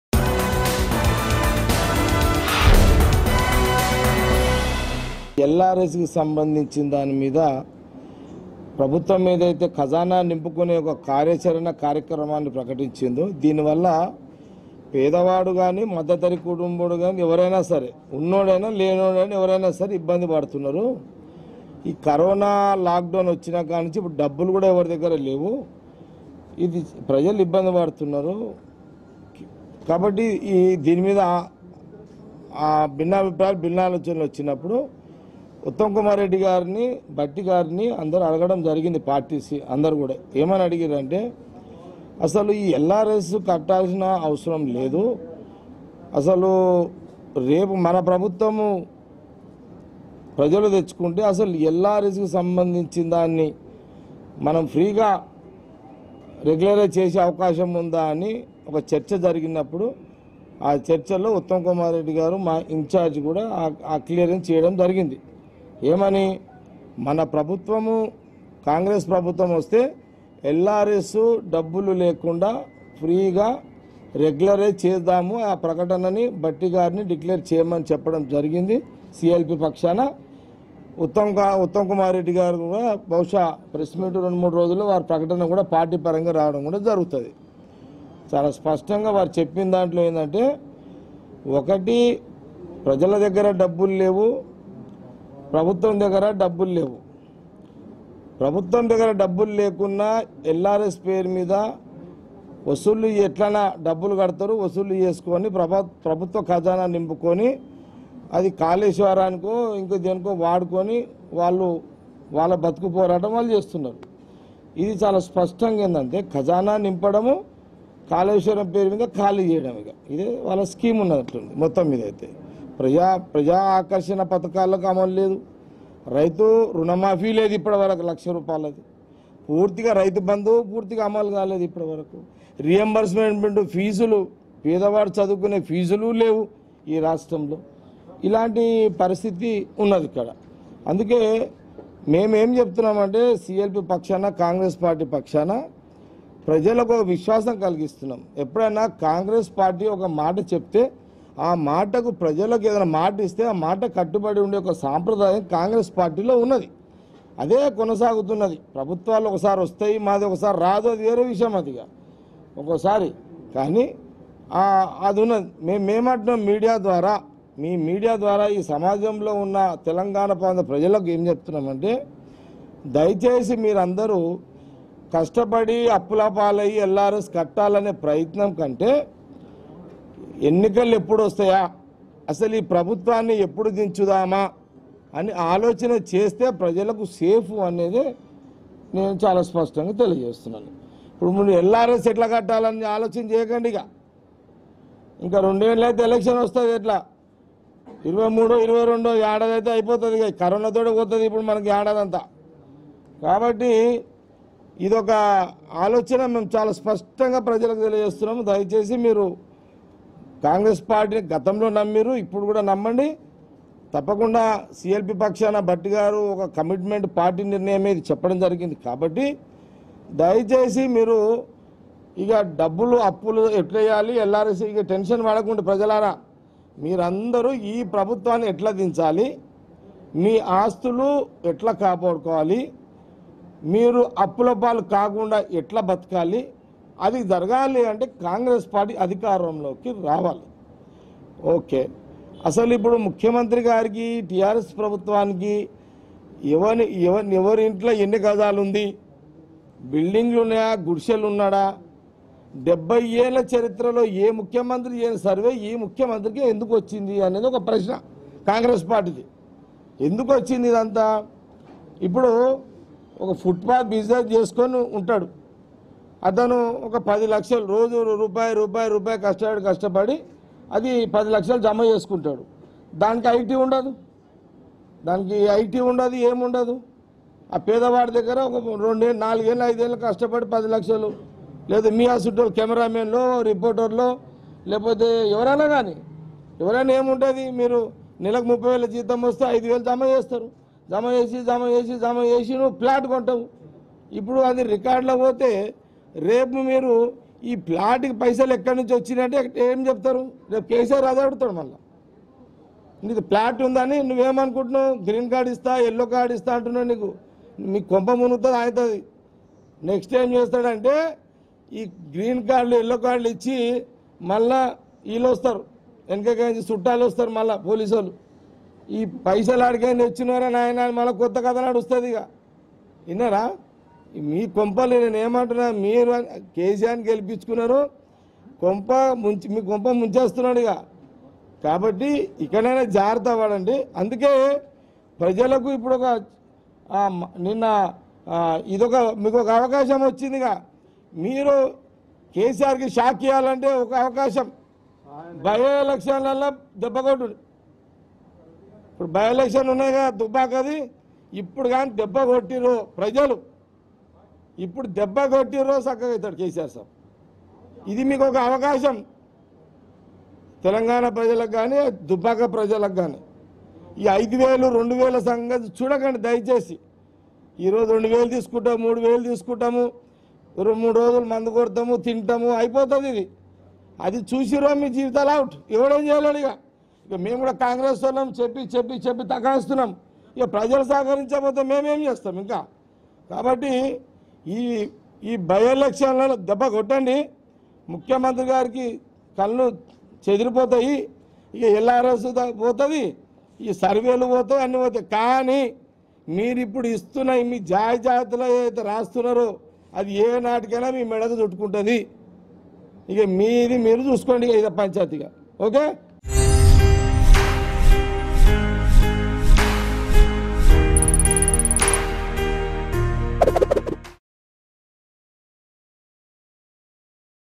Congress MLA Jagga Reddy Says To Defeat CM KCR In Dubbaka By-Election | Telangana News | Mango News
Congress MLA Jagga Reddy Says To Defeat CM KCR In Dubbaka By-Election.